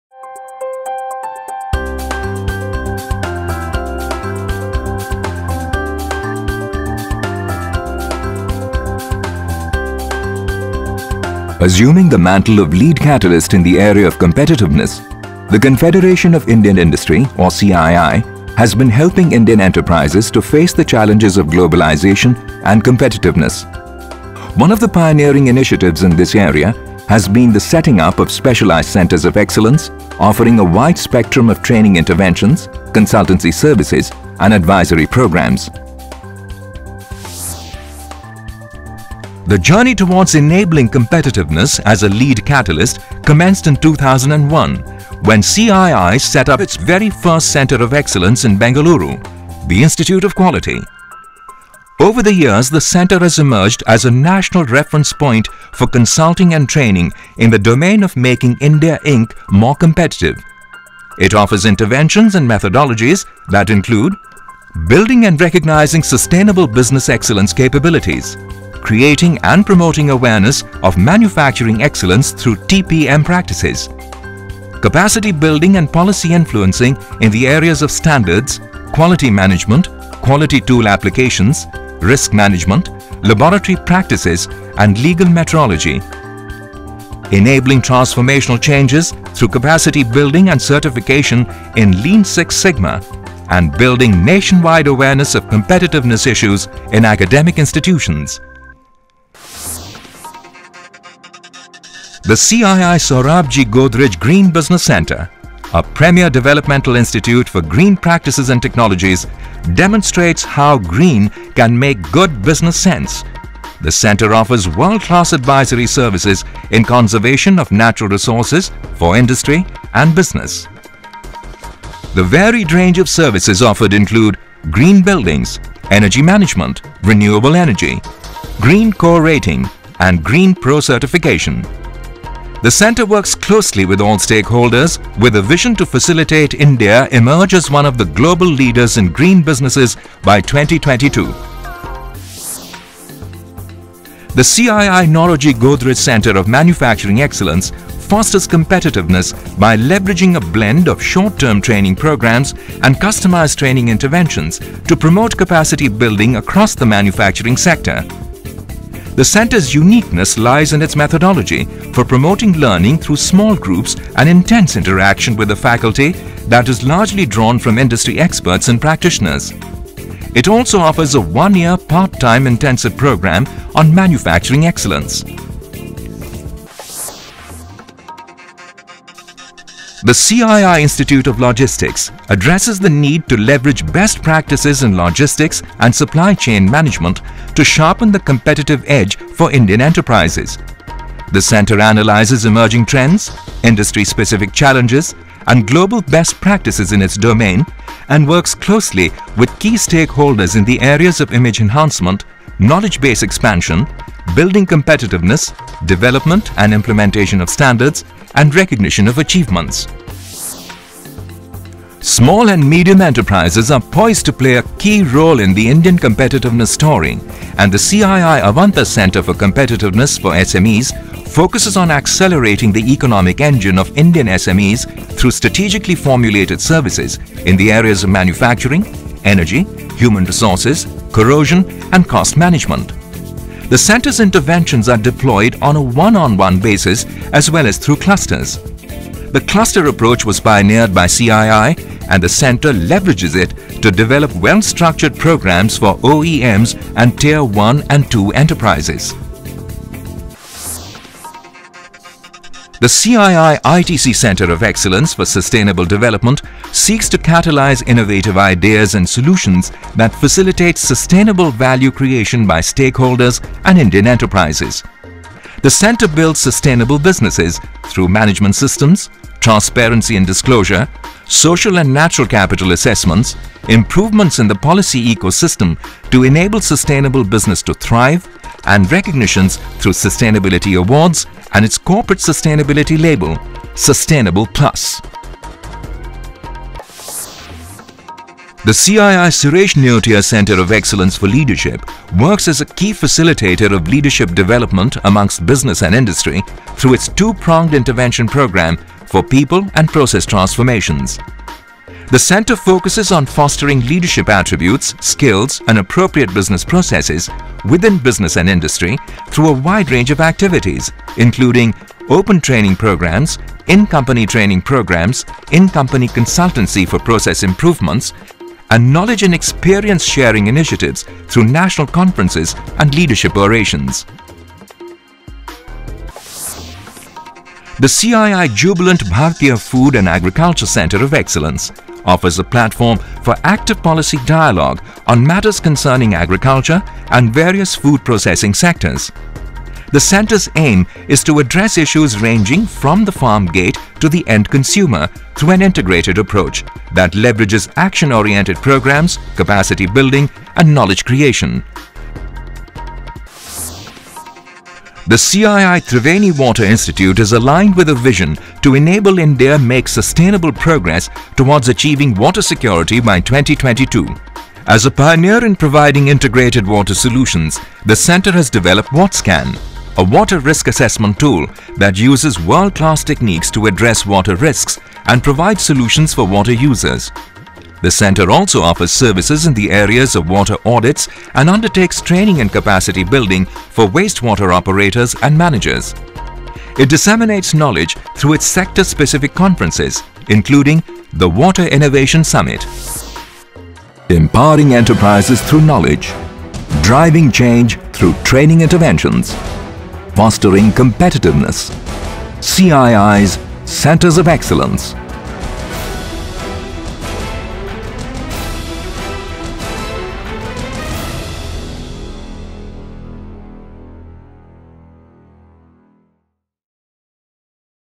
CII COEs Film 2015-16
The Confederation of Indian Industry (CII) Centres of Excellence, catalyse and facilitate to create and sustain capabilities and competitiveness of organizations with a view to impact the Indian society through training, consulting, advise and research to the industry, government  in India and Overseas.